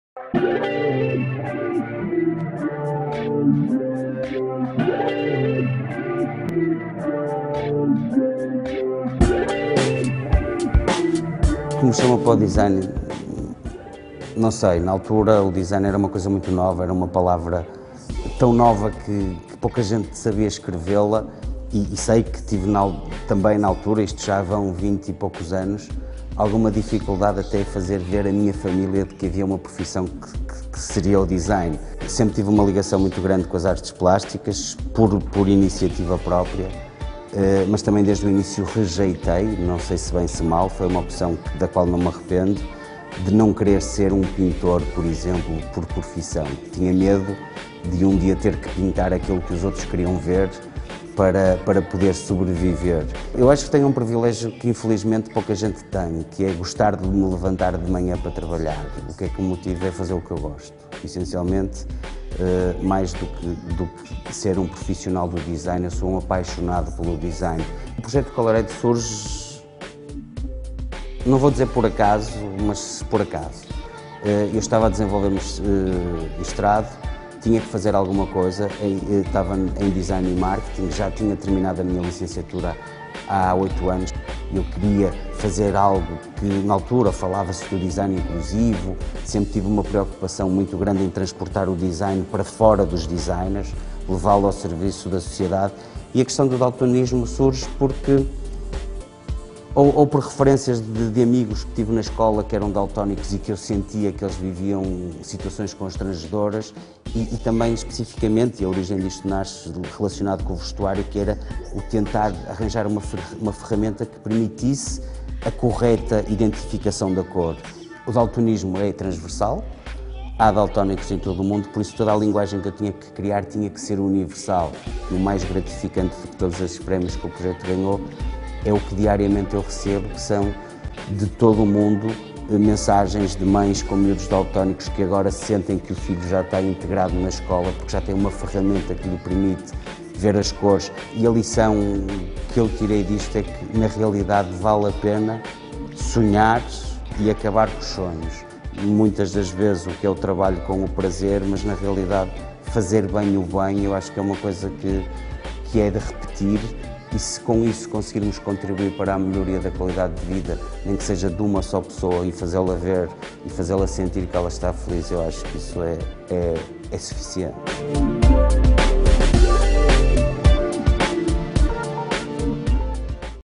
Miguel Neiva tmn vamos lá.
"Curioso desde sempre em relação ao design e às artes gráficas, cedo tomou a decisão de tentar fazer do seu trabalho algo que funcionasse em favor da sociedade. No âmbito da sua atividade desenvolveu o projeto Color Add, um sistema gráfico de identificação de cores para daltónicos cujo sucesso está já a ser reconhecido a nível mundial.

tmn
vamos lá."